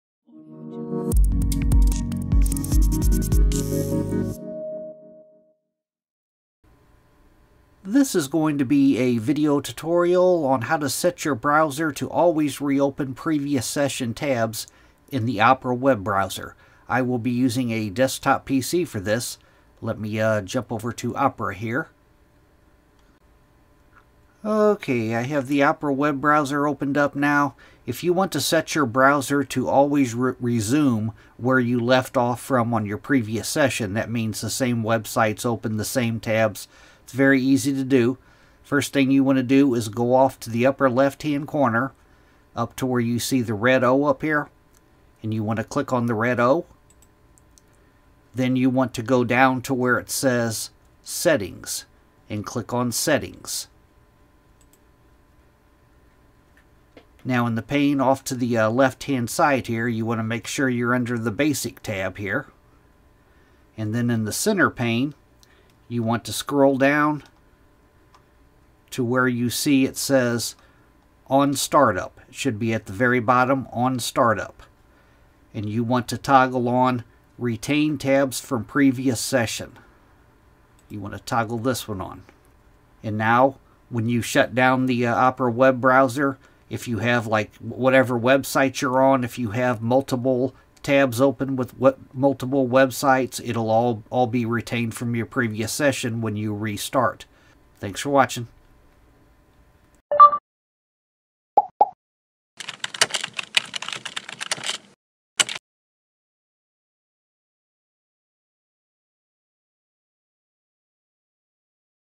How To Always Retain Previous Session Tabs On The Opera Web Browser | PC
This is a video tutorial on how to set the Opera web browser to always restore the previous session tabs. I will be using a desktop PC for this tutorial.

Simple Steps
1. Open the Opera web browser
2. Click on the red "O" in the upper left hand corner and choose "Settings".
3. In the left side pane, make sure you are on the "Basic" tab and then in the center pane, scroll down to the "Start Up" section toward the bottom.
4. Toggle on "Retain Tabs From Previous Session".

Chapters
0:00 How To Set Opera To Always Restore Previous Session Tabs
0:19 Open The Opera Browser
0:37 Click On The Red O In The Upper Left Corner And Choose Settings
0:59 Scroll Down To The Start Up Section
1:15 Toggle On Retain Tabs From Previous Session